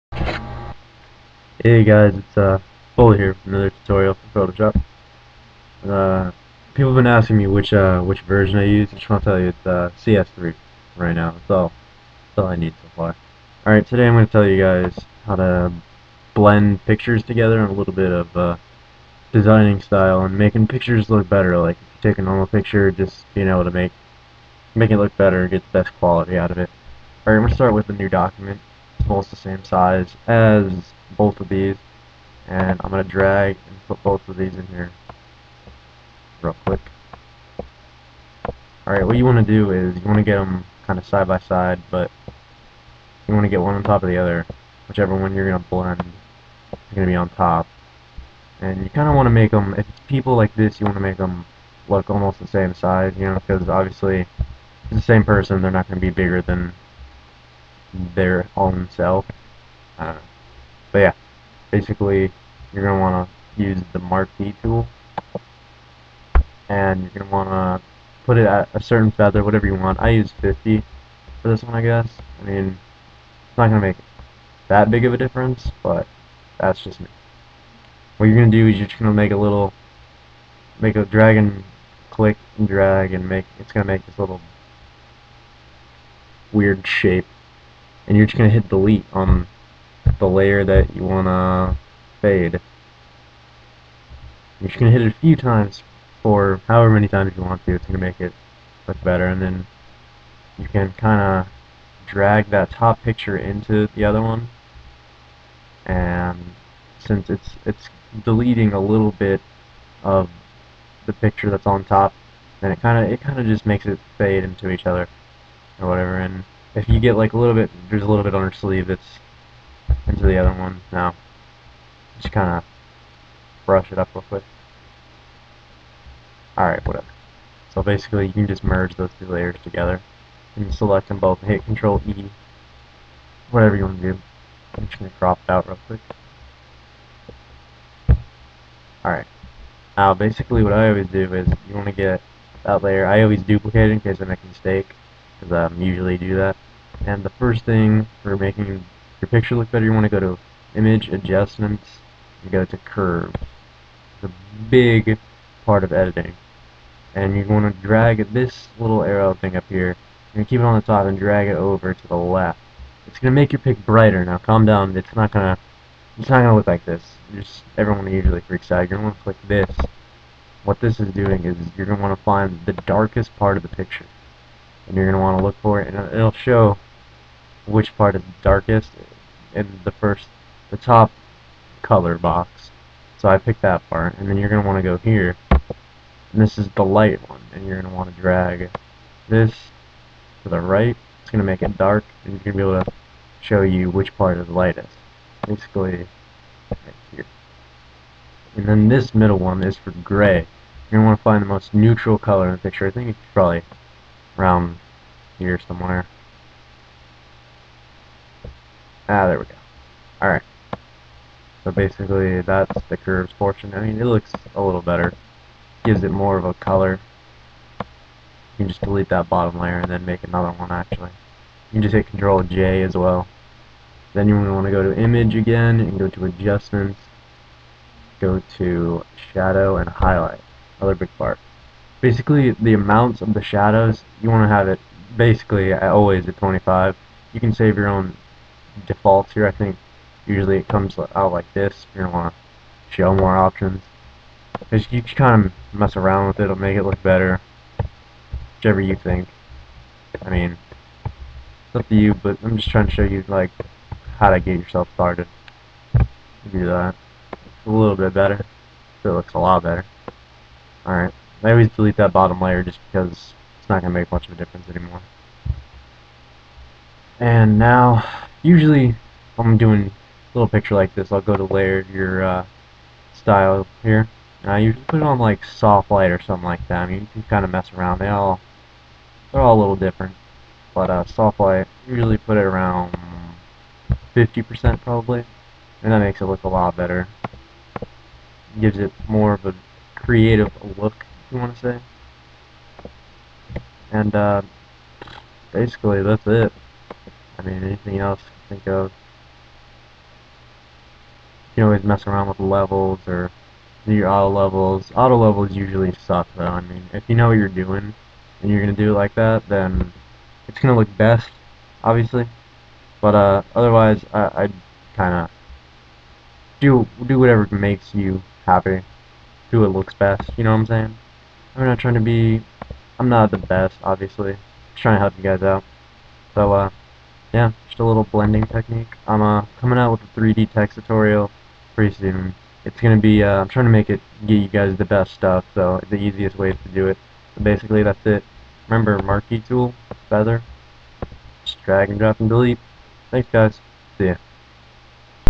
Editing/Blending Pictures in Photoshop CS3
NOTE: 2nd tutorial woot!

This is a few techniques how to blend pictures together. Also how to make any pictures look better. If i left anything out i appologize. Let me know what you think! And what else i need to cover peopele!
-Foley